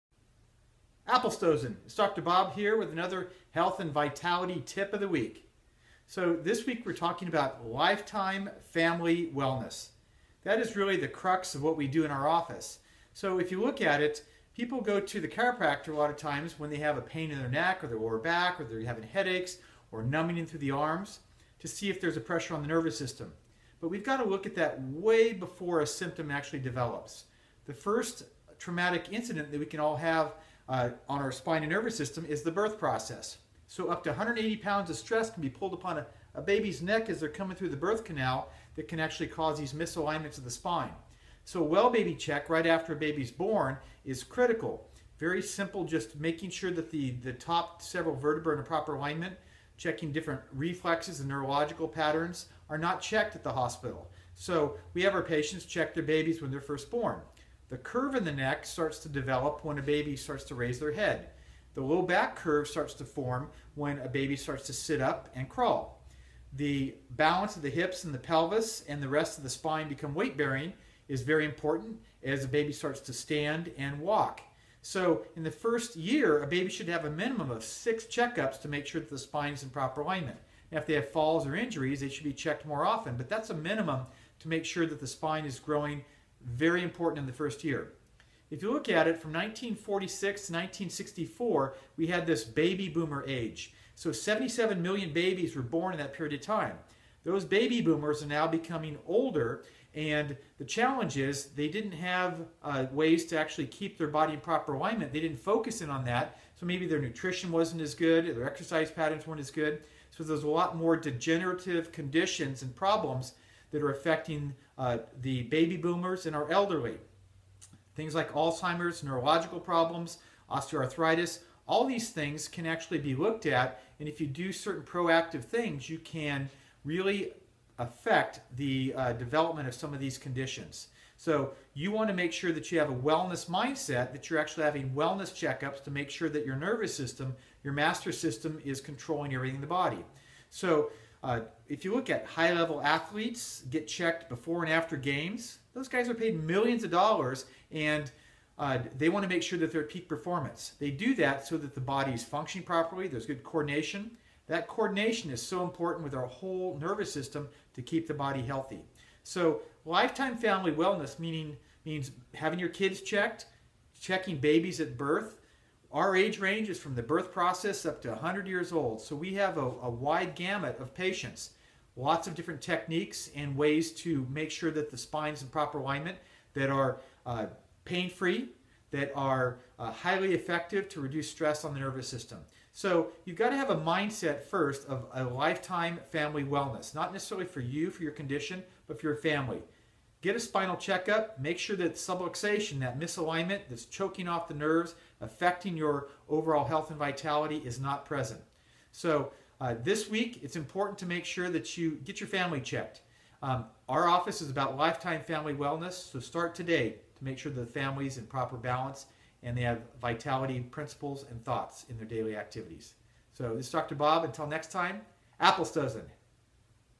Dr. Robert Graykowski, Lifetime Family Wellness!
The average living age of Americans increases every year. During the "baby boomer" era approximately 77 million babies were born in the United States during the boom years of 1946 to 1964. In 2011, the oldest from this era will turn 65, and on average can expect to live to 83. Many will continue well into their 90s. Most people in the U.S. continue to retain the habits they developed as children and teenagers. For many Americans, these habits included lack of regular exercise, sedentary activities, and poor nutrition.

As adults we no longer possess the free pass we had when we were kids.